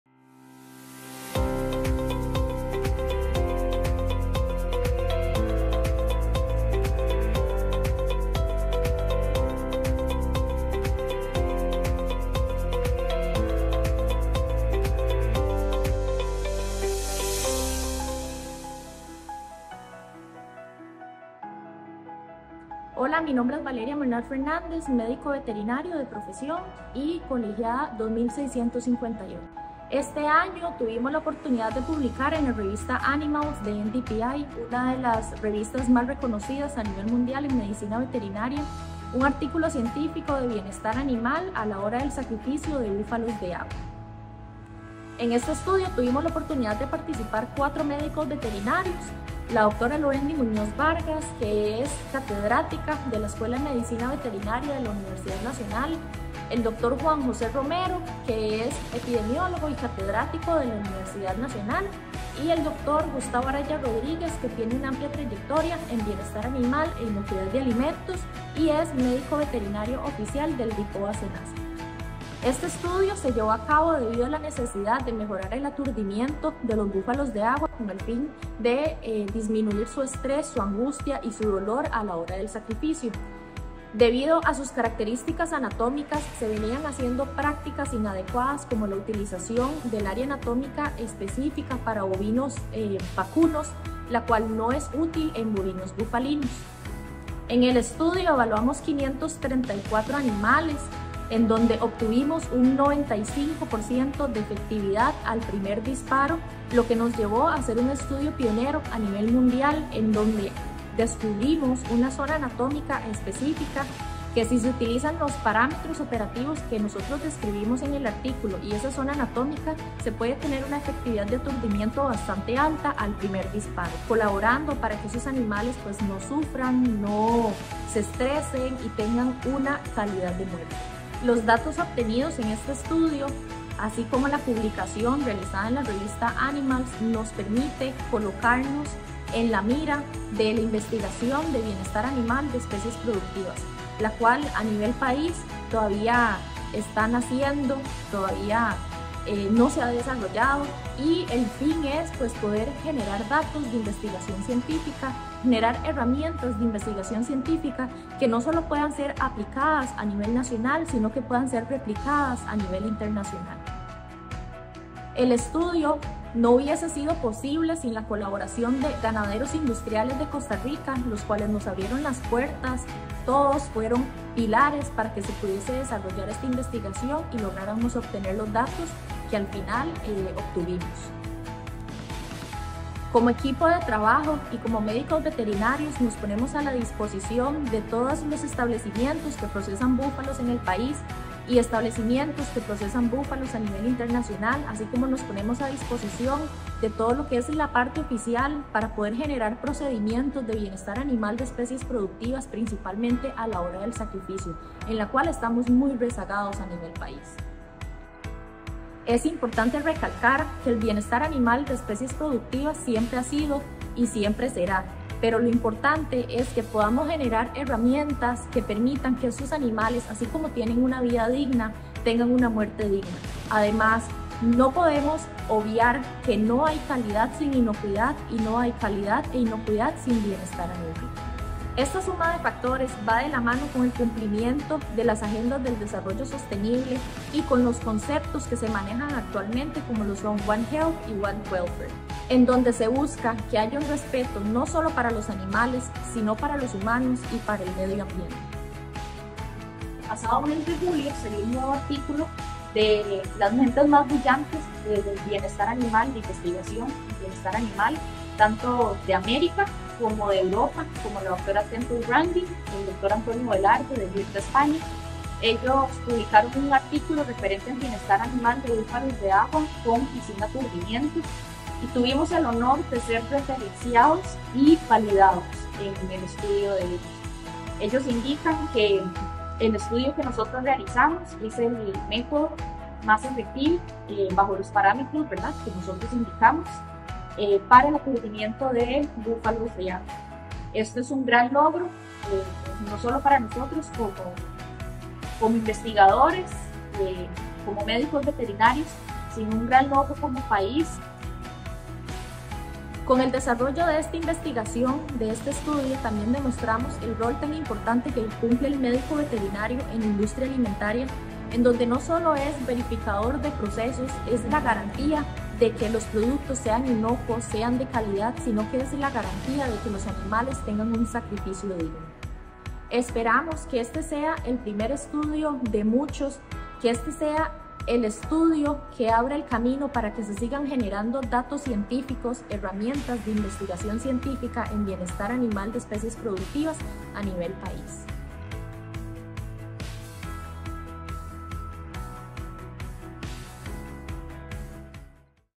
Vanguardia Veterinaria: "Aturdimiento efectivo al primer disparo en Búfalos de Agua"
Estudio realizado por la Dra. Valeria Molnar, la Dra. Lohendy Muñoz, el Dr. Juan José Romero y el Dr. Gustavo Araya.

El estudio se llevó a cabo debido a la necesidad de mejorar el aturdimiento de búfalos de agua, disminuir su estrés, su angustia y su dolor a la hora del sacrificio.
Un aspecto importante es que este estudio fue publicado como artículo pionero a nivel mundial en bienestar animal en la revista Animals MDPI, una de las revistas de mayor prestigio en el mundo. Incluso se publicó además un nuevo artículo de las mentes más brillantes del mundo, en el que se refiere este estudio por la reconocida Dra. Temple Grandin, declarada una de las 100 personas más influyentes del mundo y toda una vida dedicada al bienestar animal, así como el Dr. Antonio Velarde de España y Dr. Daniel Mota.